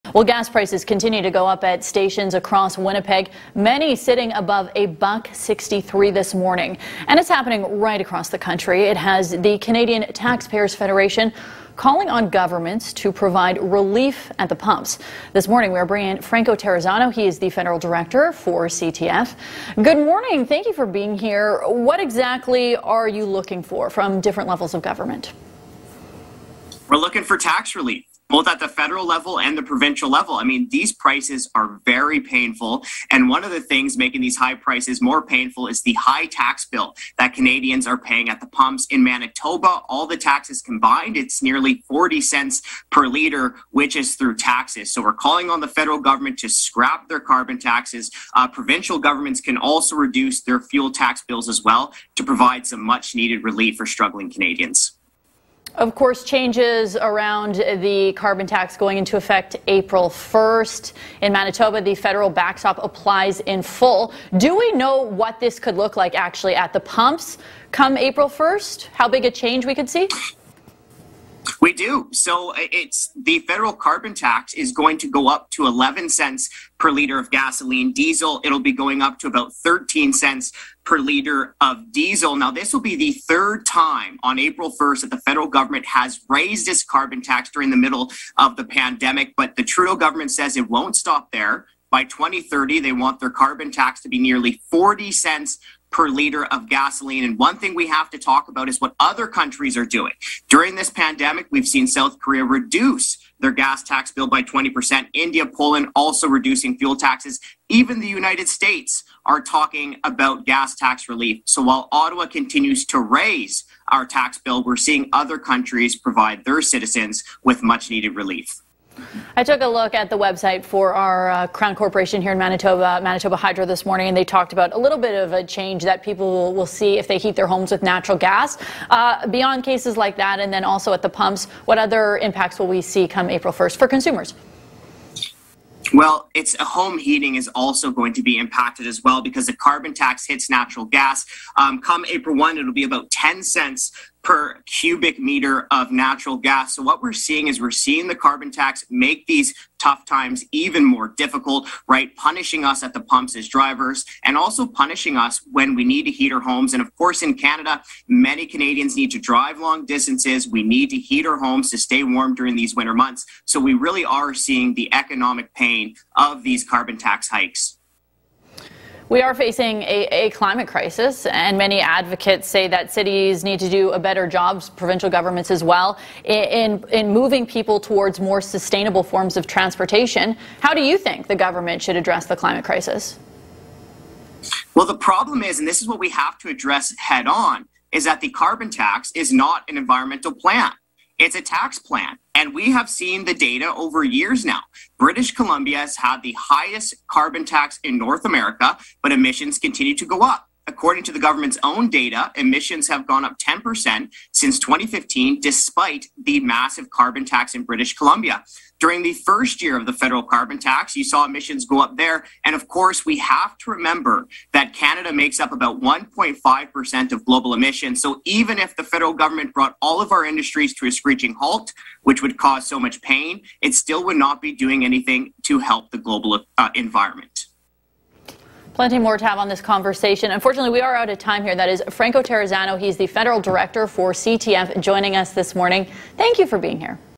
Trudeau needs to scrap the carbon tax.
The Canadian Taxpayers Federation is calling on the federal and provincial governments to immediately provide tax relief as prices at the pump soar.